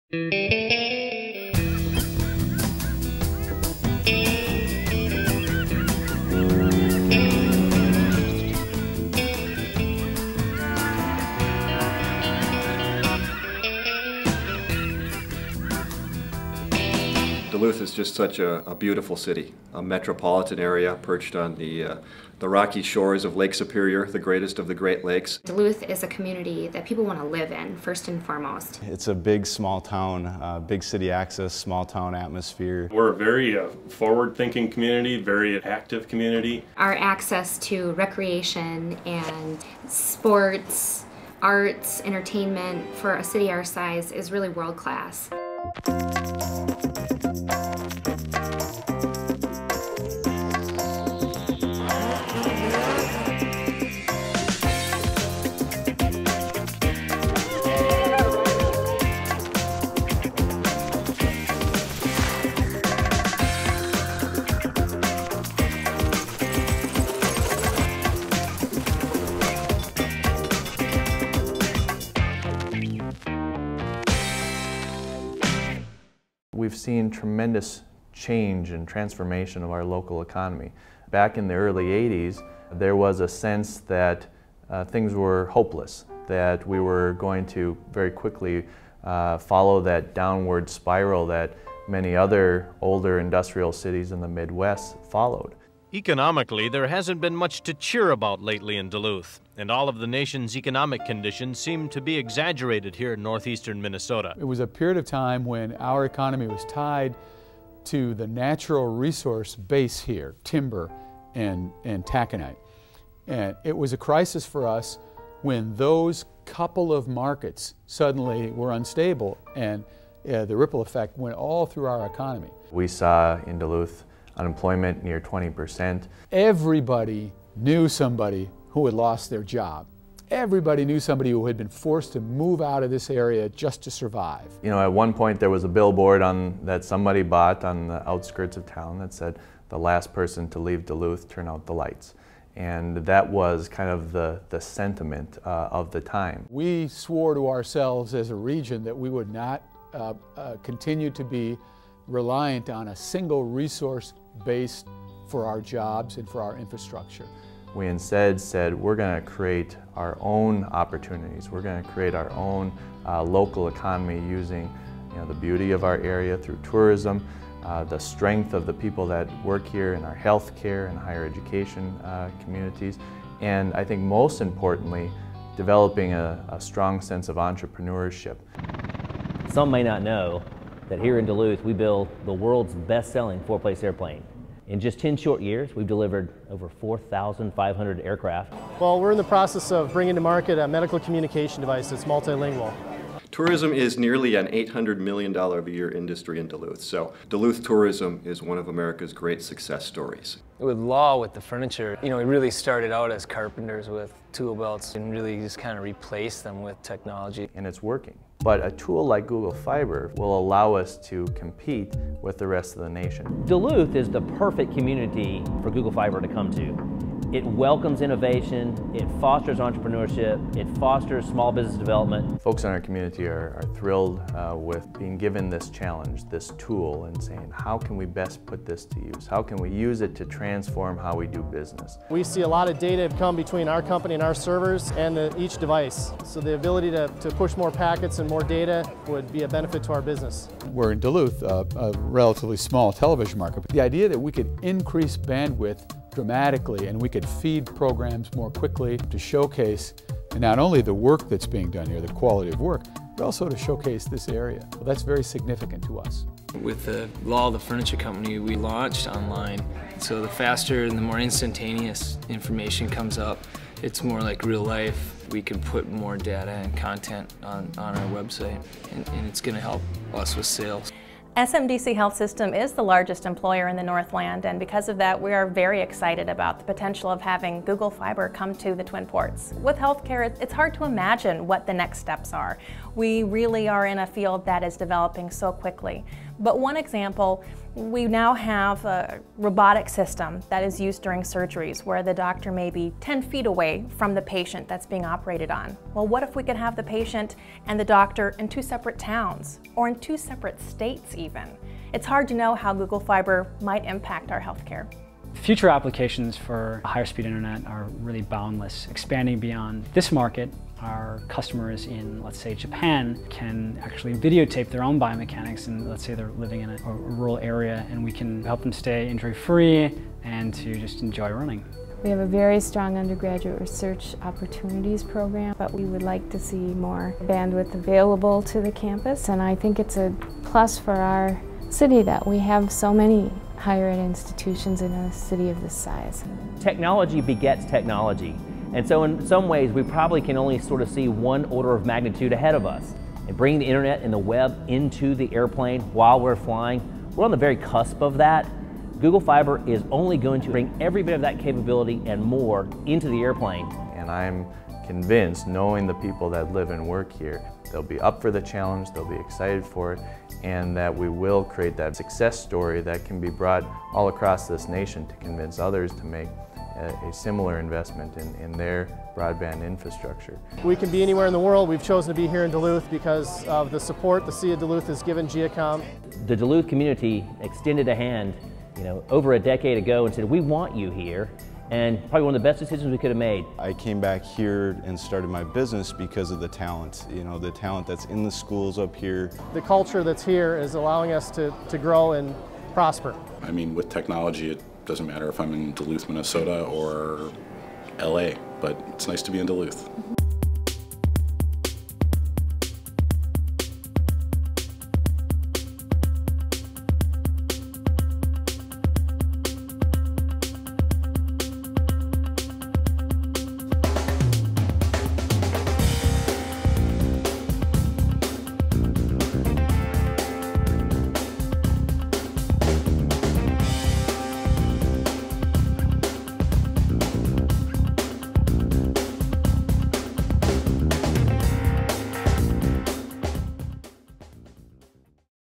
Duluth: Portal to the World (A Google Fiber Documentary)
Duluth is a city with an incredible story. After weathering a devastating economic downturn in the 1980s, Duluth reinvented itself as an innovator in eco-tourism, general aviation, medical communications and renewable energy. See how this city on the shore of Lake Superior could be poised for even greater success. And learn what Google fiber could mean for Duluth.